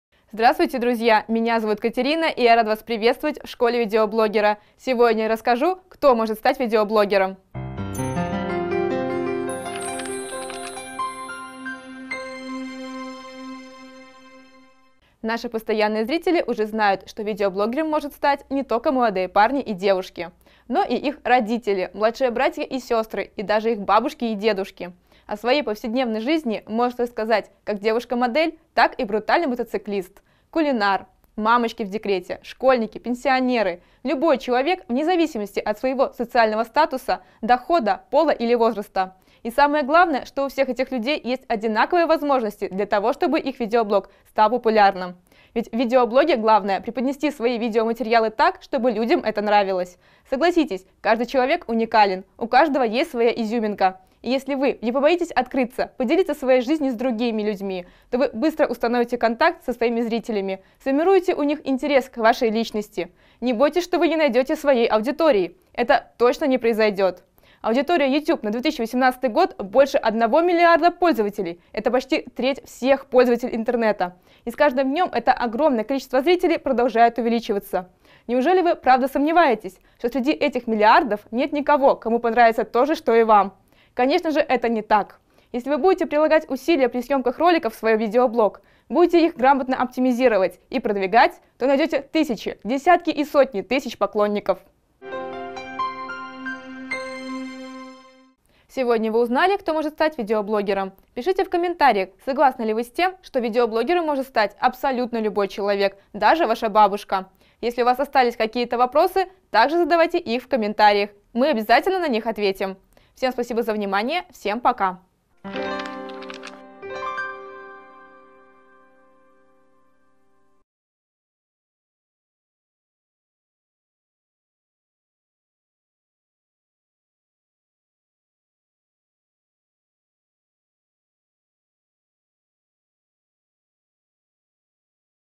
Кто может стать видеоблогером? Смотрите видео, где мы расскажем, можете ли вы стать видоблогером!
Наши постоянные зрители уже знают, что видеоблогером может стать не только молодые парни и девушки от 18 до 25 лет, но и их родители, младшие братья и сёстры и даже их бабушки и дедушки! О своей повседневной жизни может рассказать как девушка модель, так и брутальный мотоциклист, кулинар, мамочки в декрете, школьники, пенсионеры. Любой человек в независимости от своего социального статуса, дохода, пола или возраста. И самое главное, что у всех этих людей есть одинаковые возможности для того, чтобы их видеоблог стал популярным. Ведь в видеоблогинге главное преподносить свои видеоматериалы так чтобы людям это нравилось. Согласитесь, каждый человек уникален, у каждого есть своя изюминка - и если вы не побоитесь открыться, поделиться своей жизнь с другими людьми, то вы быстро установите контакт со своими зрителями, сформируете у них интерес к вашей личности. Не бойтесь что вы не найдете своей аудитории - этого точно не произойдёт! Аудитория YouTube на 2018 год больше одного
миллиарда пользователей – это почти треть всех пользователей Интернета, и с каждым днём это огромное количество зрителей продолжает увеличиваться! Неужели вы правда сомневаетесь что среди этих миллиардов не никого кому нравилось бы то же что и вам? Конечно же это не так! Если вы будете прилагать усилия при съёмках роликов в свой видеоблог, будете их грамотно оптимизировать и продвигать, то найдёте тысячи, десятки и сотни тысяч поклонников.